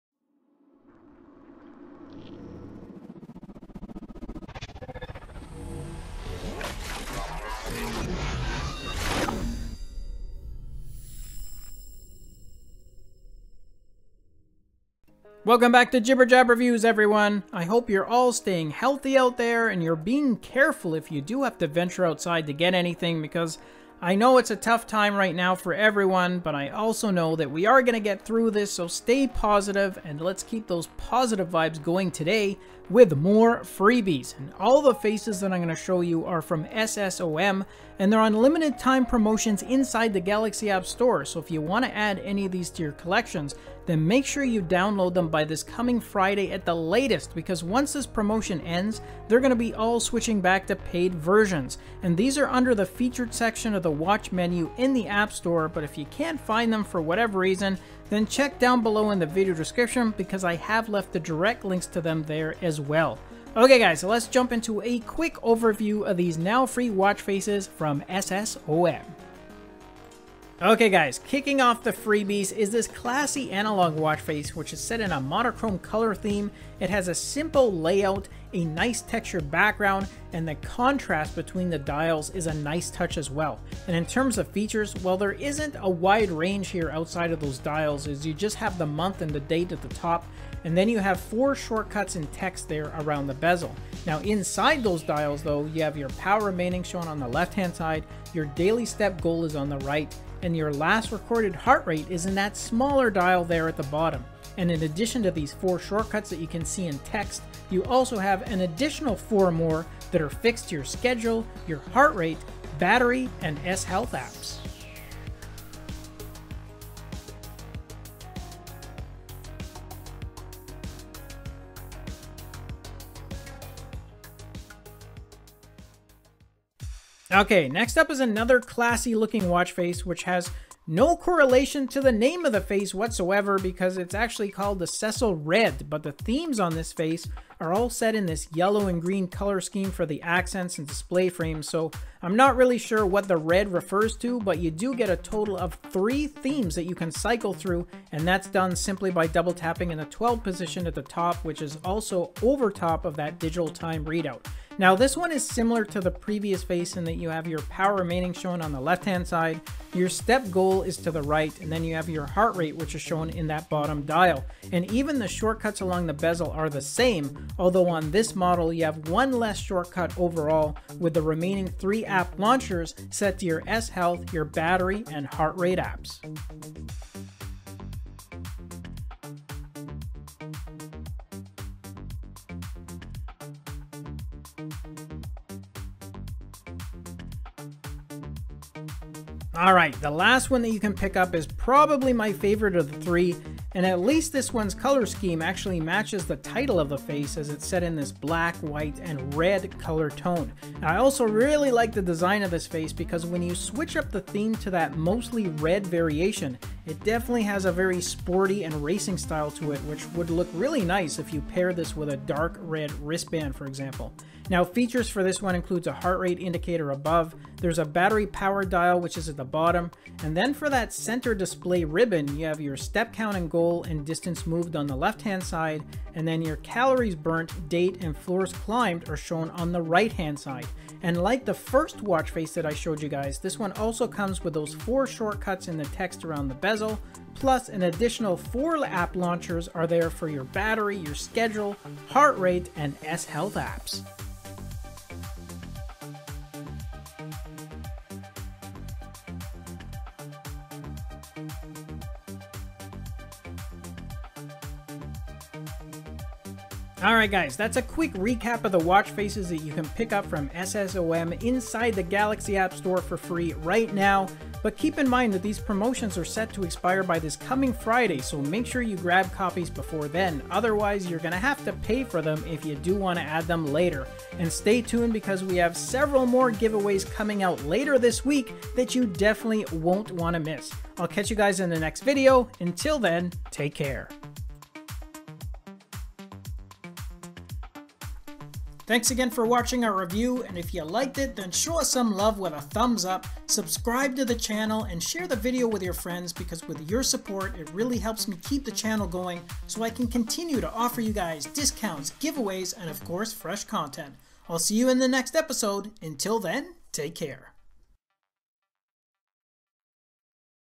*FREEBIE ALERT!* Samsung Galaxy Watch Active 2/Galaxy Watch Faces by SSOM
Grab these 3 watch faces for free but only until this coming Friday and then they will be switching back to paid versions.  Hurry!

Samsung Watchfaces
Smartwatch